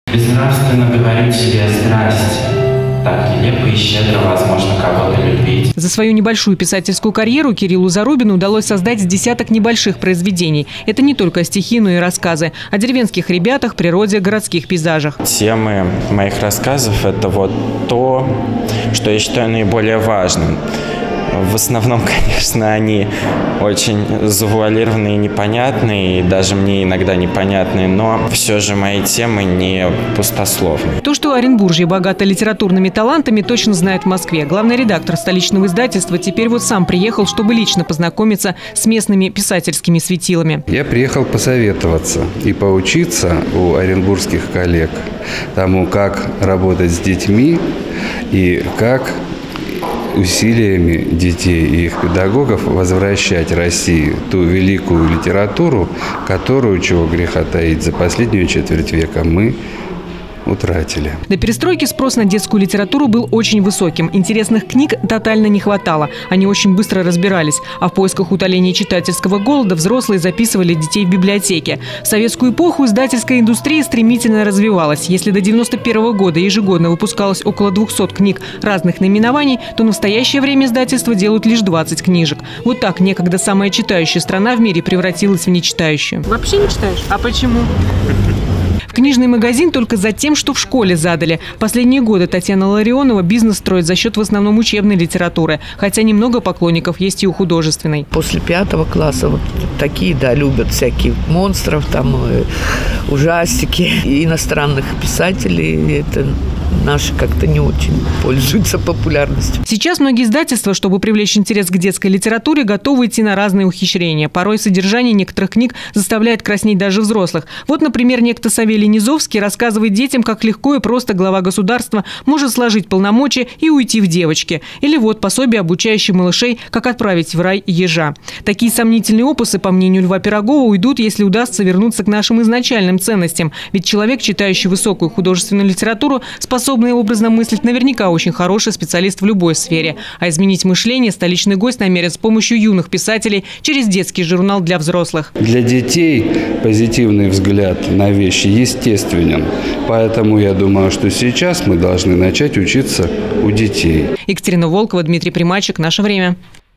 В Оренбурге обсудили проблемы детской литературы
Произведения оренбургских детских писателей напечатают в Москве. Сегодня столичные издатели лично пообщались со школьниками, чьим увлечением является литература. Встреча прошла в областной библиотеке имени Крупской, где школьники презентовали свои стихи и рассказы. Но только чтением общение не закончилось, затронули и тему кризиса детской литературы. Сегодня магазины, по мнению экспертов, переполнены книгами низкого содержания. Да и молодежь не особо стремится провести время за чтением. Можно ли возродить детскую литературу выясняла Екатерина Волкова.